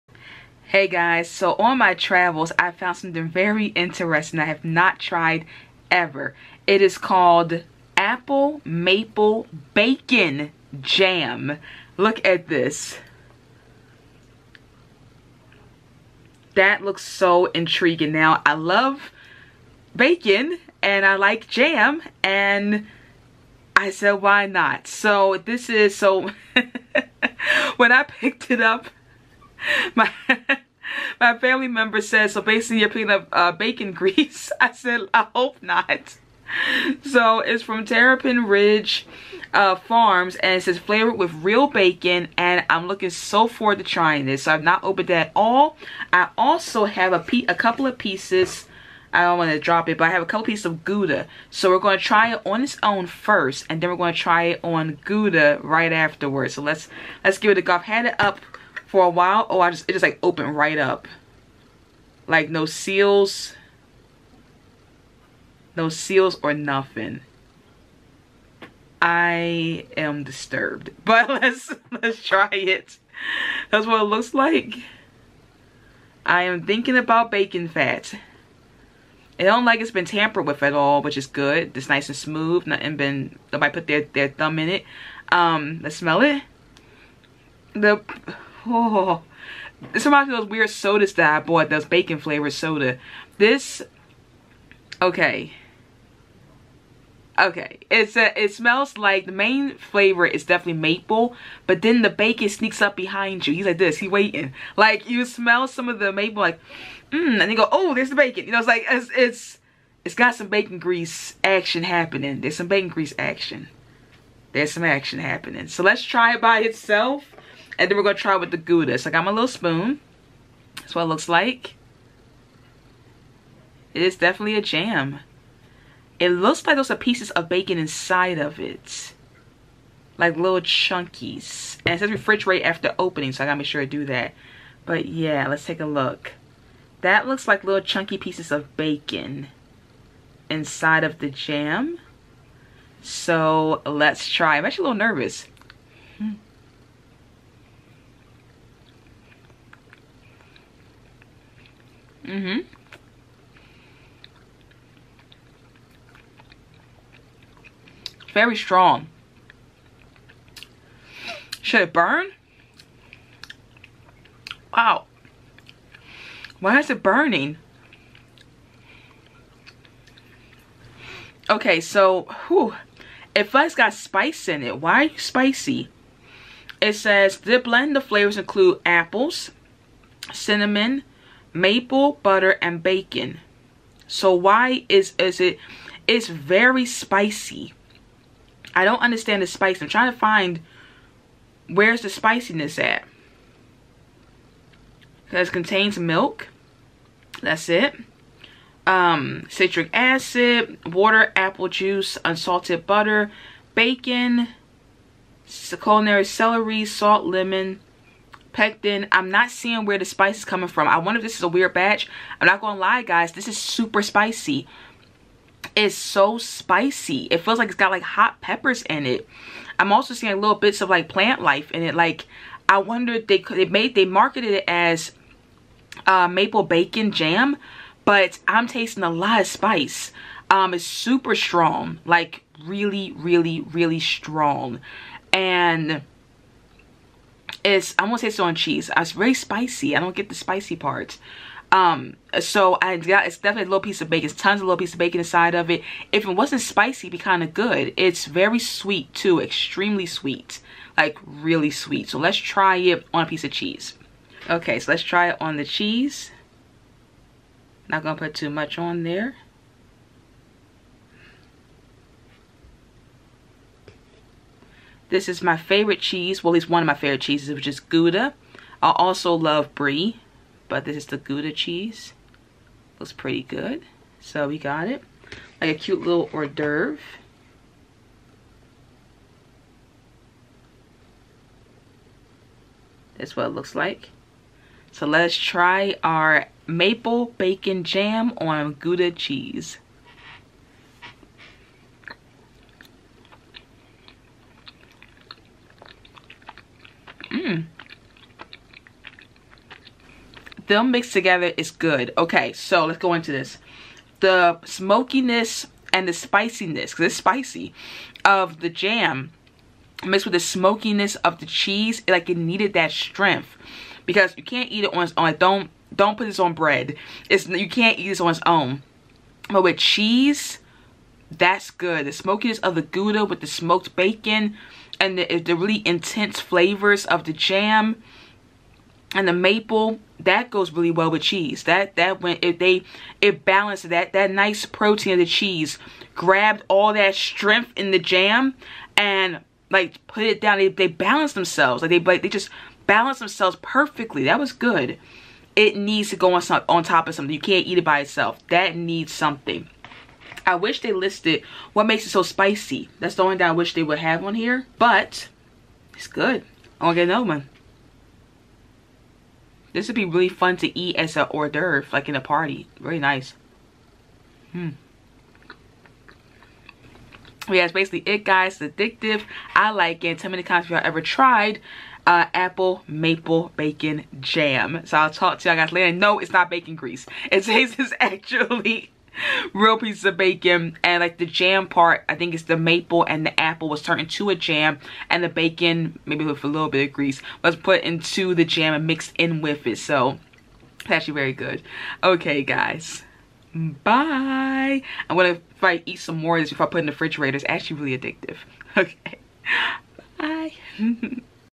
Terrapin Ridge Farms: Apple Maple Bacon Jam - Review 🥓🥞🍎
Hey guys, so I found some bacon flavored jam at the store! Let's see how it taste!